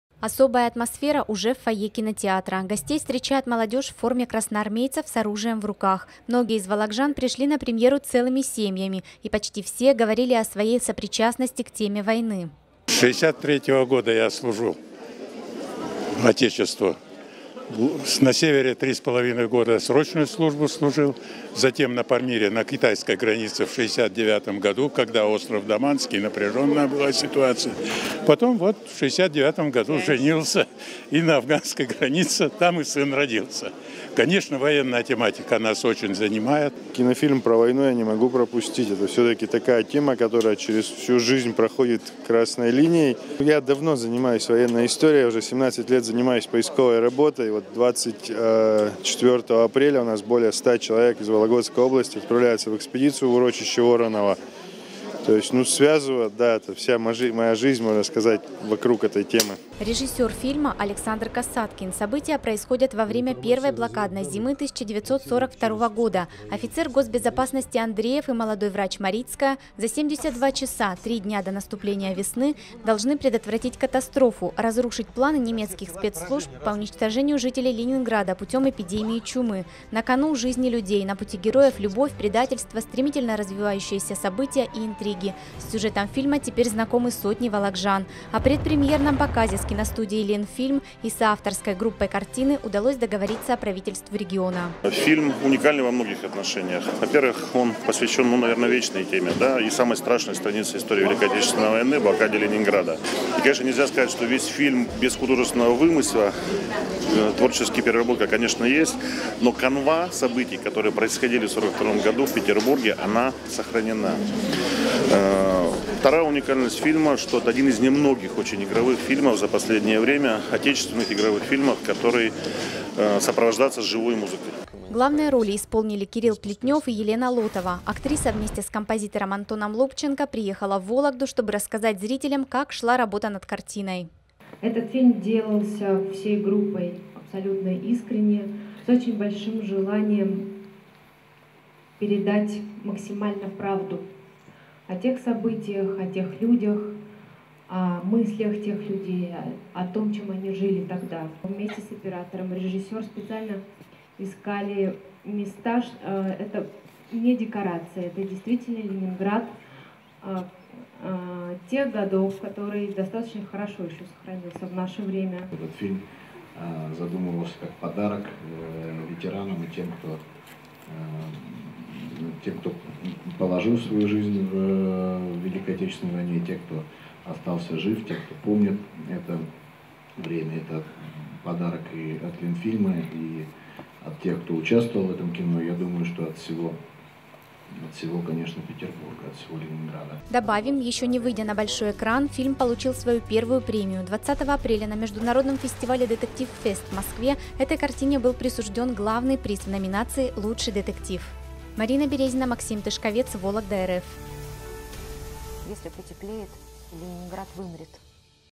Предпремьерный показ военной драмы «Три дня до весны» с аншлагом прошел в Вологде
21 апреля, в кинотеатре «Ленком» прошел бесплатный показ художественной картины «Три дня до весны». Фильм о блокаде Ленинграда, снятый режиссером Александром Касаткиным, вологжане увидели раньше других зрителей. Ведь в кинопрокат драма выходит только 4 мая. О предпремьерном показе с киностудией «Ленфильм» и соавторской группой картины удалось договориться Правительству региона.